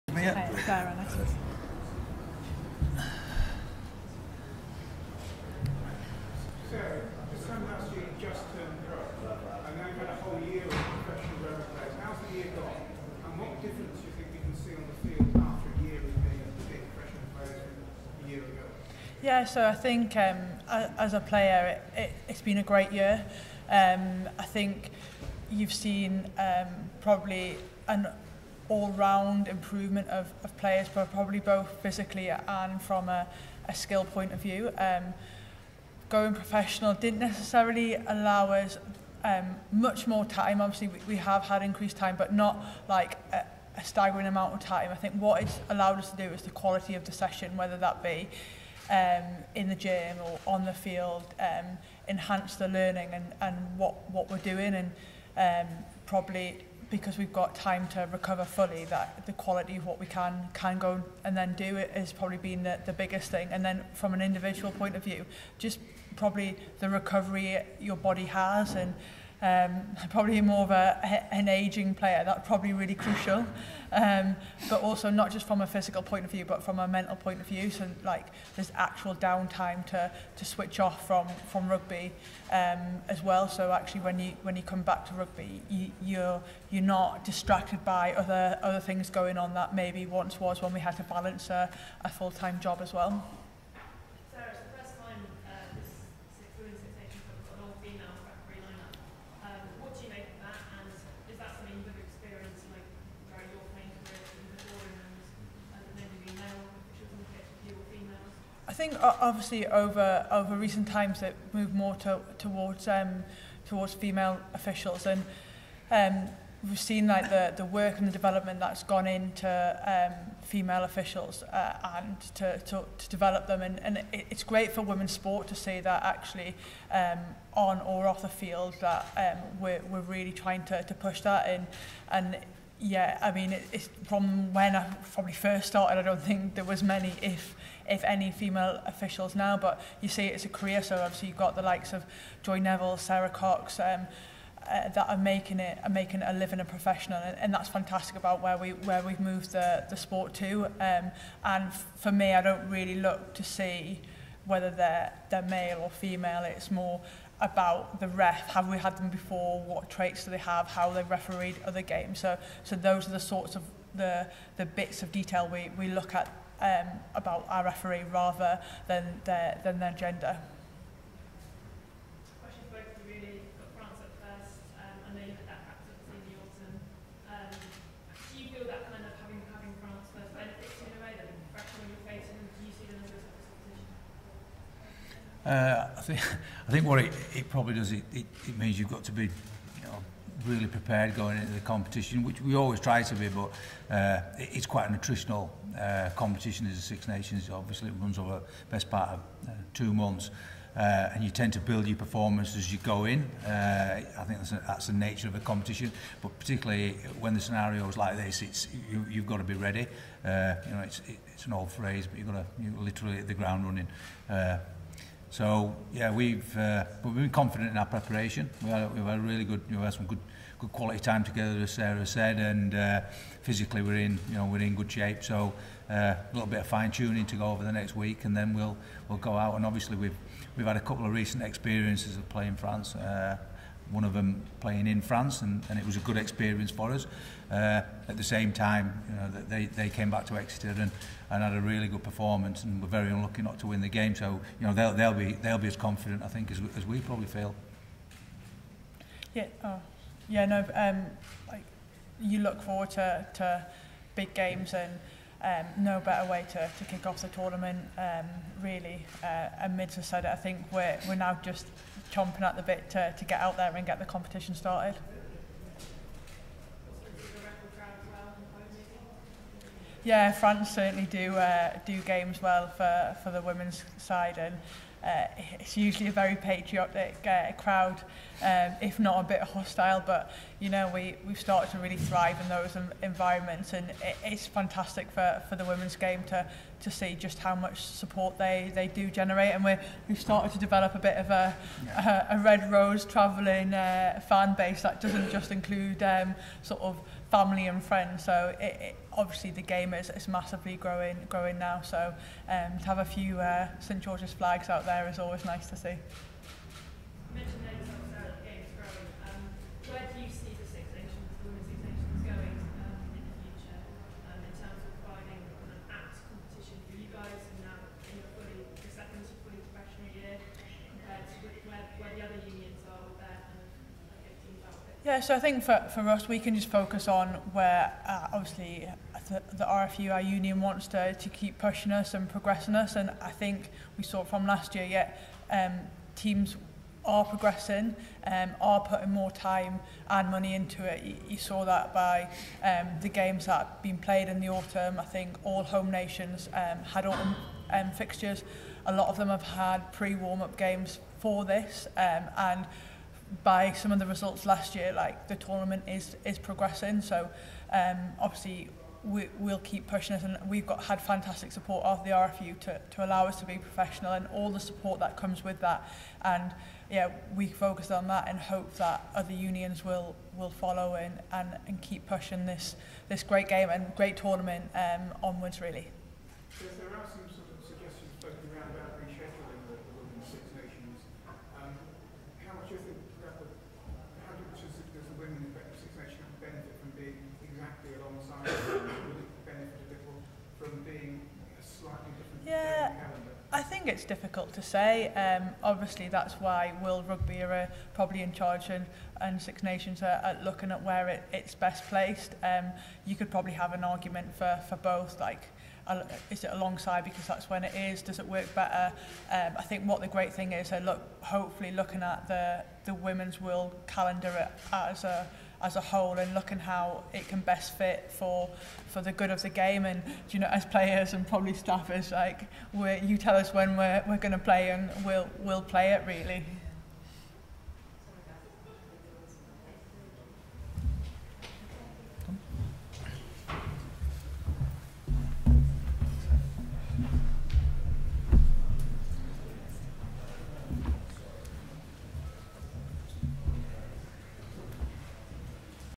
England Women's Six Nations 2020 Launch - FULL Press Conference
The head coach of Ireland’s Women Six Nations squad, Simon Middleton and captain Sarah Hunter take questions about the tournament and competition ahead of the championship next month.

The press conference took pack at London’s Tobacco Dock as part of the official launch of the Guinness Six Nations tournament alongside the men’s teams.

Thanks